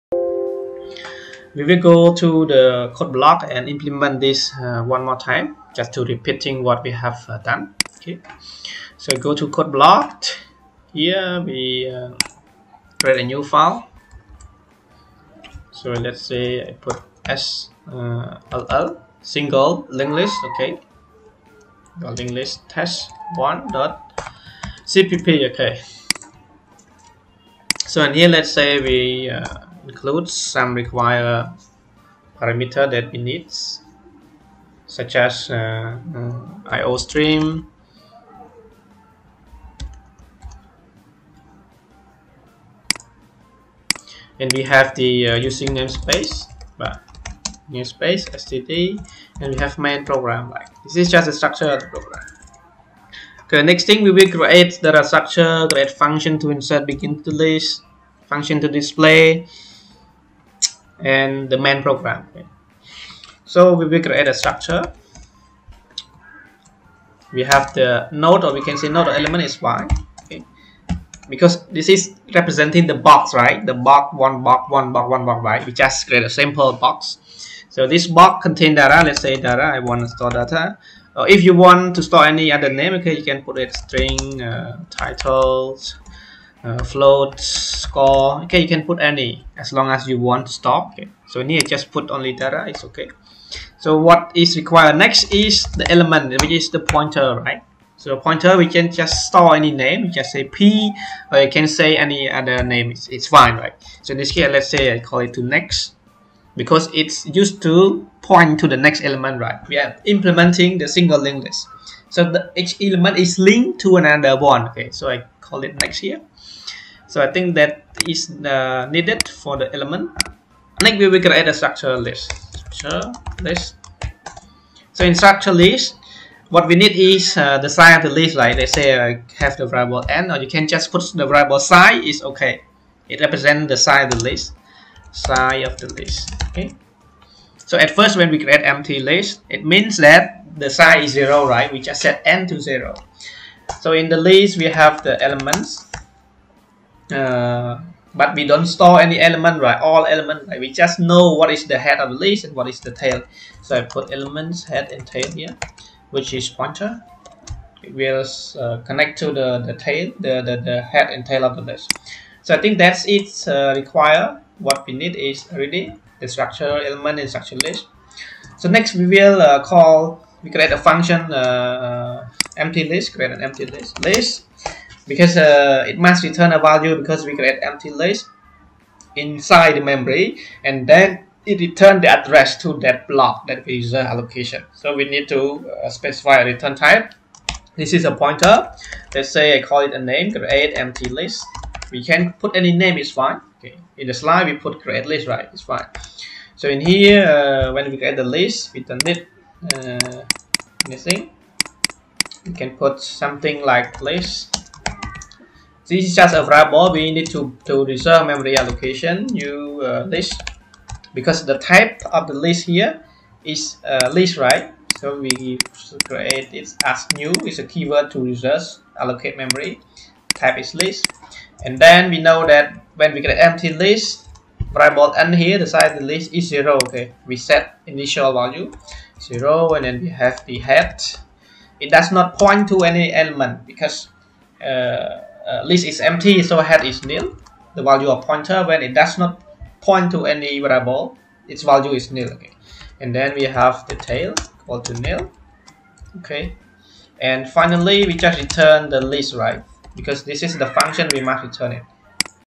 03 Coding create structure and empty list
Coding example in C++ to define structure and empty list.